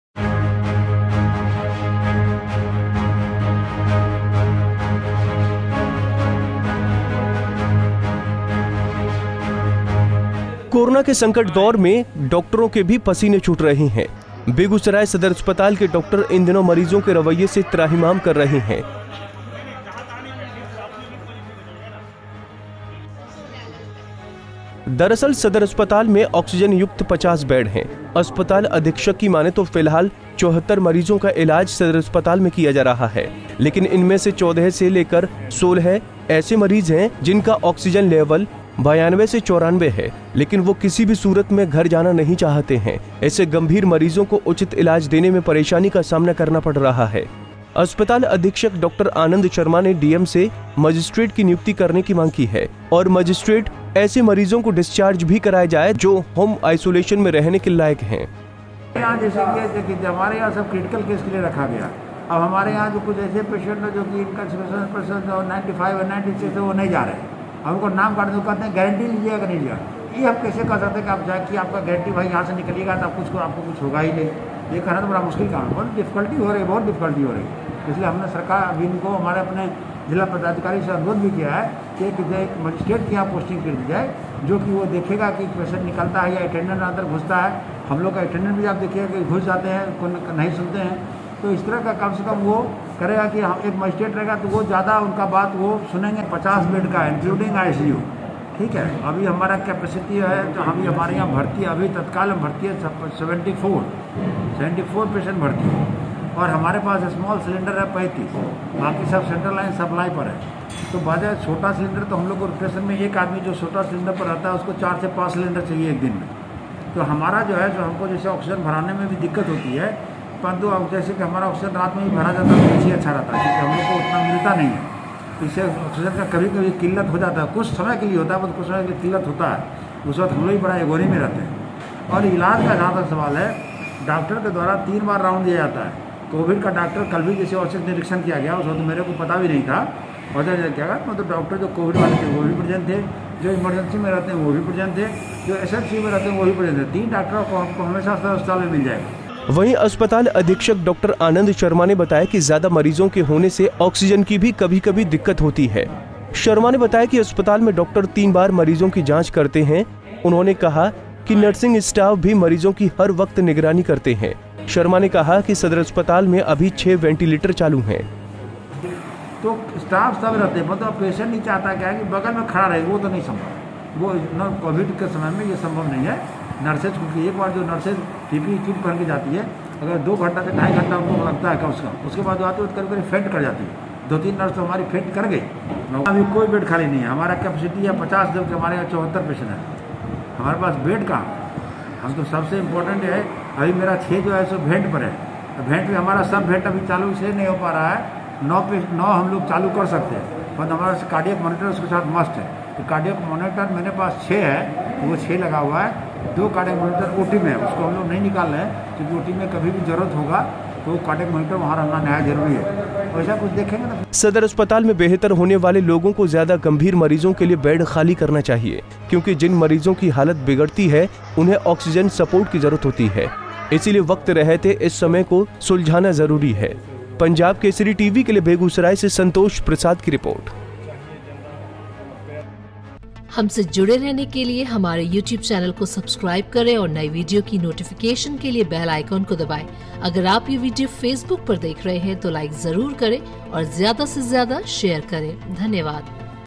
बिहार में कोरोना मरीजों ने डॉक्टरों के नाक में कर दिया है दम
कोरोना संकट के दौर में डॉक्टरों के भी पसीने छूट रहे हैं. बेगूसराय सदर अस्पताल के डॉक्टर इन दिनों मरीजों के रवैये से त्राहिमाम कर रहे हैं. दरअसल सदर अस्पताल में ऑक्सीजन युक्त 50 बेड हैं. अस्पताल अधीक्षक की माने तो फिलहाल 74 मरीजों का इलाज सदर अस्पताल में किया जा रहा है. लेकिन इनमें से 14 से लेकर 16 ऐसे मरीज हैं जिनका ऑक्सीजन लेवल 92से 94 हैं. लेकिन वह किसी भी सूरत में घर जाना नहीं चाहते हैं. इससे गंभीर मरीजों को उचित इलाज देने में परेशानी का सामना करना पड़ रहा है. अस्पताल अधीक्षक डॉक्टर आनंद शर्मा ने डीएम से मजिस्ट्रेट की नियुक्ति करने की मांग की है. और मजिस्ट्रेट ऐसे मरीजों को डिस्चार्ज भी कराया जाए जो होम आइसोलेशन में रहने के लायक हैं.